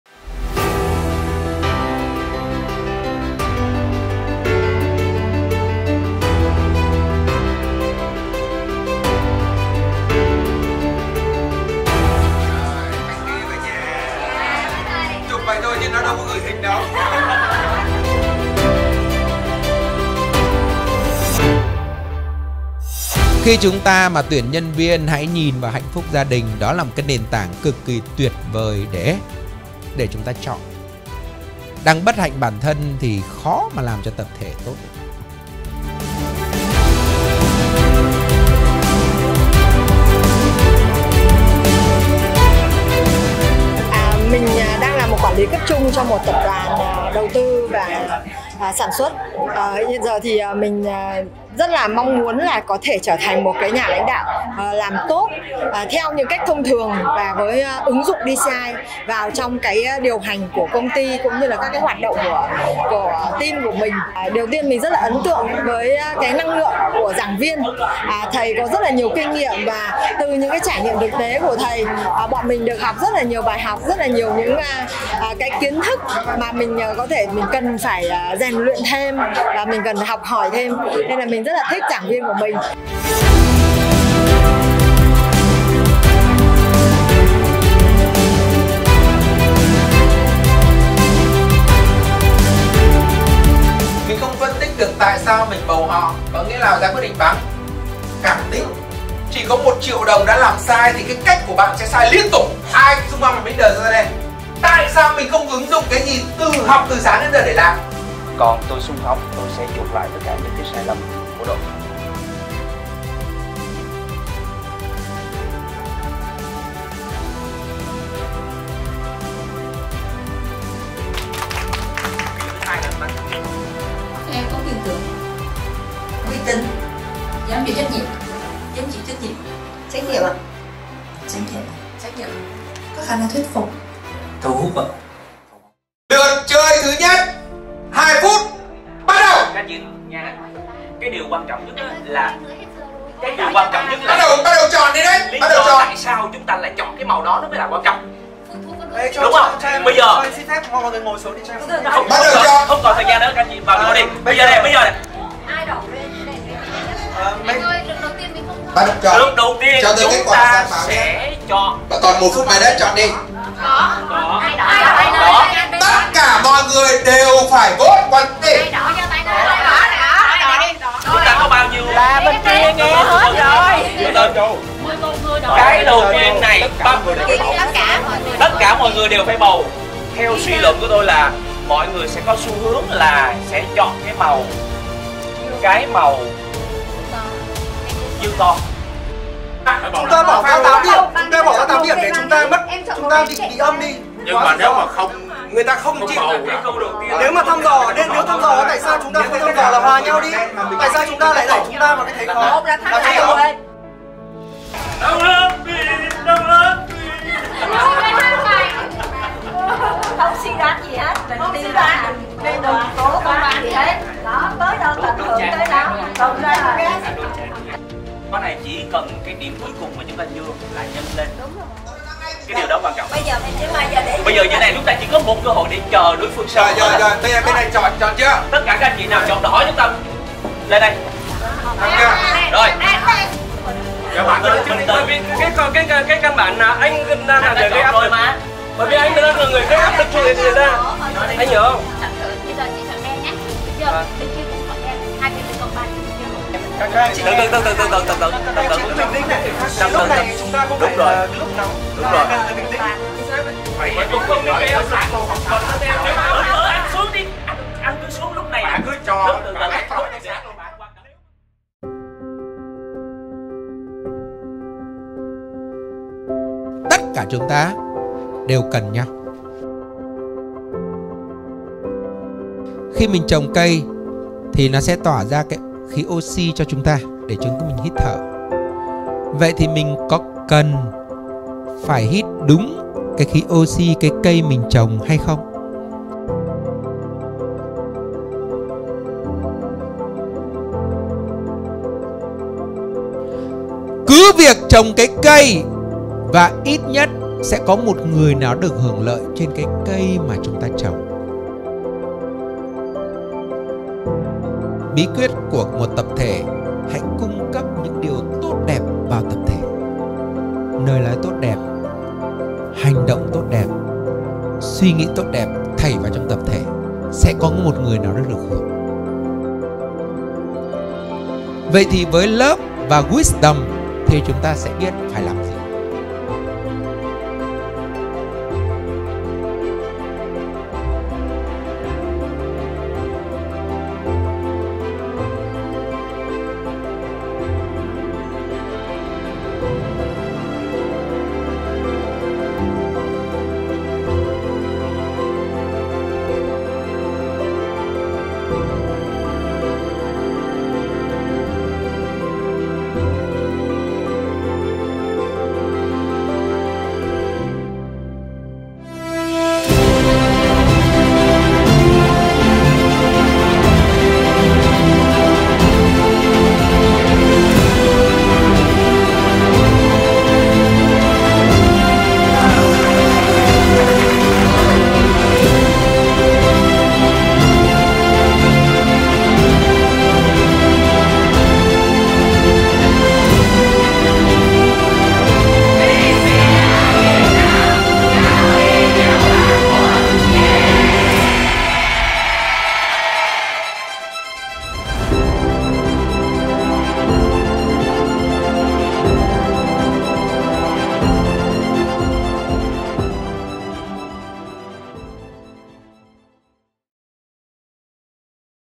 NĂNG ĐOẠN KIM CƯƠNG CẤP ĐỘ 8: AUTOMATIC LEADERSHIP - NHÀ LÃNH ĐẠO GIẢ KIM| DCI Việt Nam
Ngày 28-29/10/2023 vừa qua đã diễn ra chương trình Năng Đoạn Kim Cương Cấp độ 8: "AUTOMATIC LEADERSHIP - NHÀ LÃNH ĐẠO GIẢ KIM" vô cùng sôi nổi, máu lửa & không kém phần lắng đọng. DCI Việt Nam thân mời mọi người cùng xem lại những khoảnh khắc tuyệt vời tại chương trình này!!!

Give What You Want - Trao Đi Điều Bạn Muốn,
DCI Việt Nam
➨ THEO DÕI KÊNH DCI VIỆT NAM ĐỂ LUÔN ĐƯỢC CẬP NHẬT NHỮNG VIDEO MỚI NHẤT